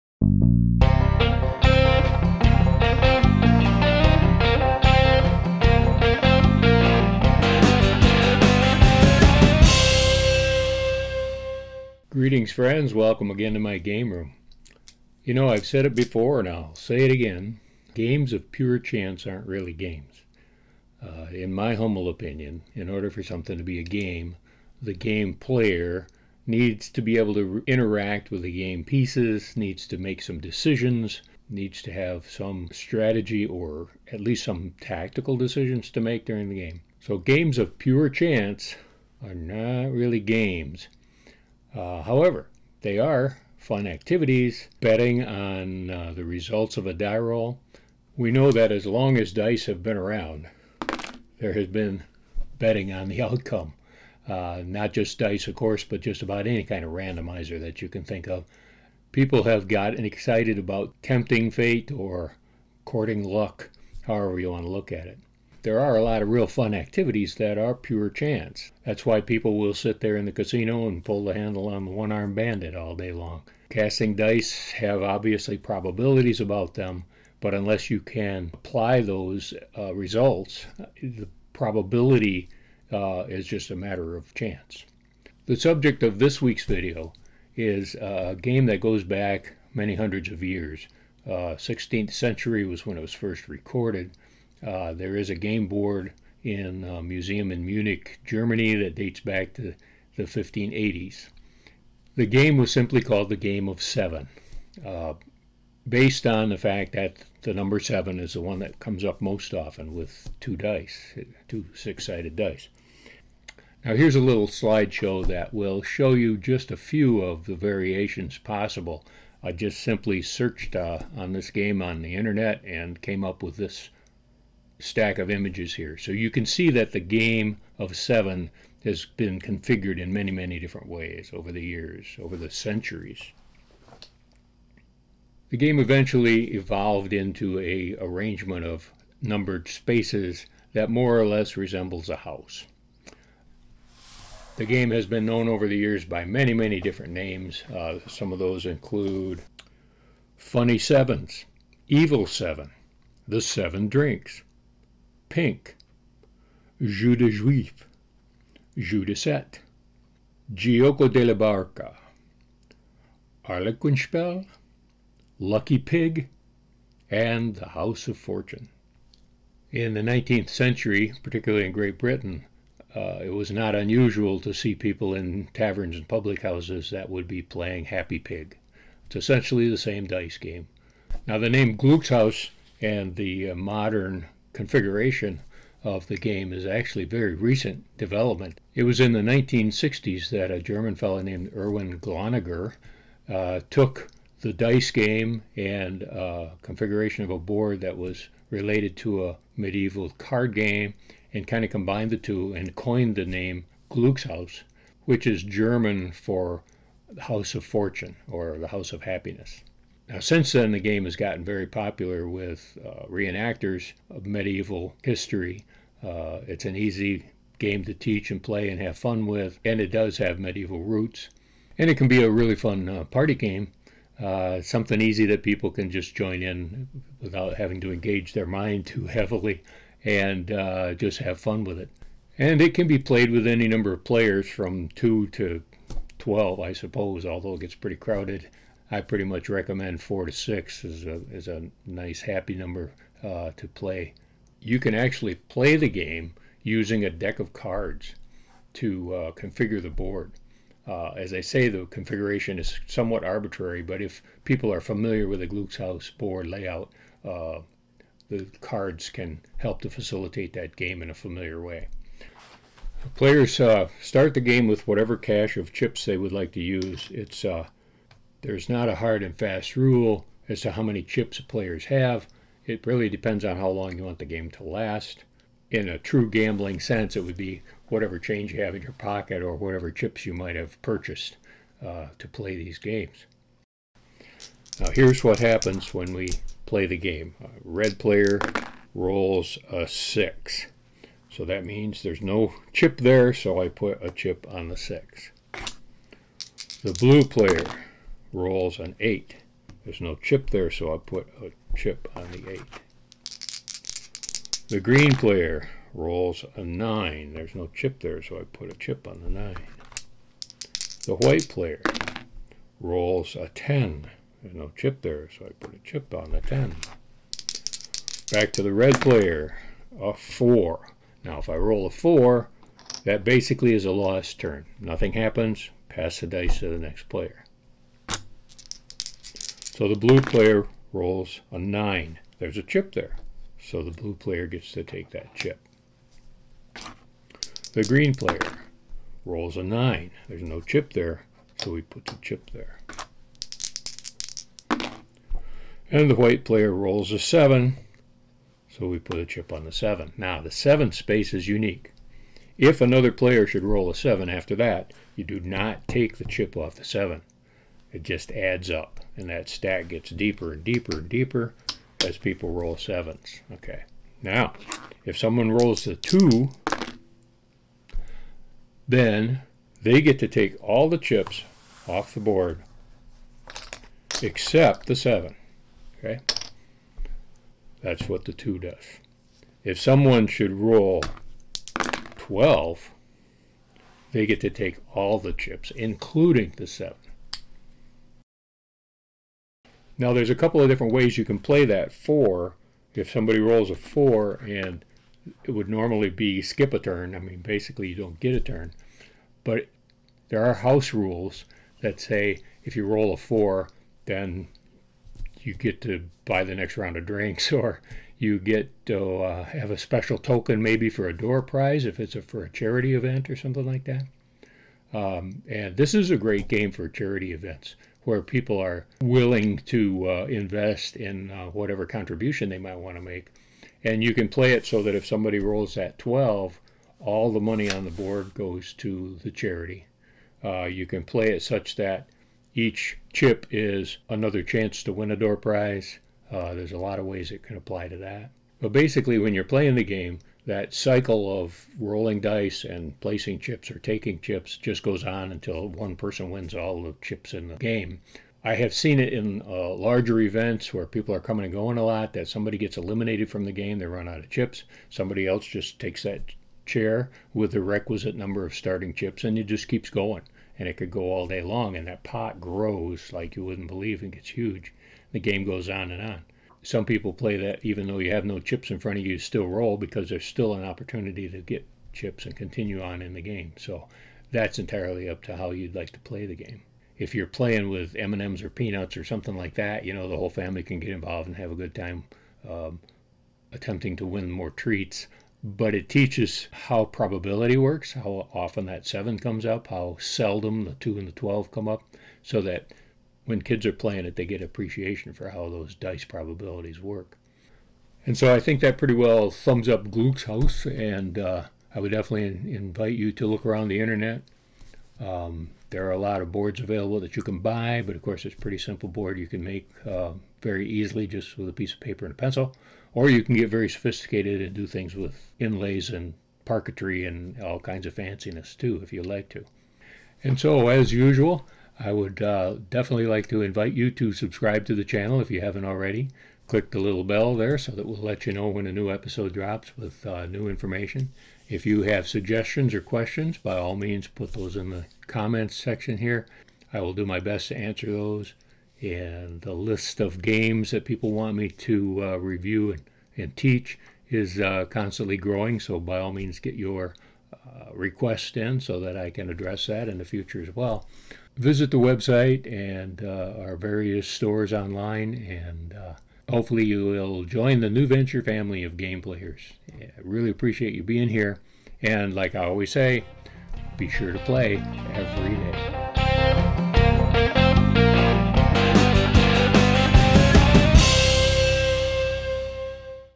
Gluckshaus is a Medieval dice game from central Europe. Will the dice roll in your favor?
It wasn't called "Gluckshaus" until the 20th century, but over 500 years ago this little gambling game was first played in central Europe. It's known as Jeu du Sept, Gioco della Barca, Arlequinspiel, and in England as "Lucky Pig."

It's not much of a game, but simple a matter of luck, but the game adapts well to a party atmosphere. I've seen adaptations of the game at charity events that really get people involved in the games "push your luck" aspect (like most gambling games).

...and be sure to play every day!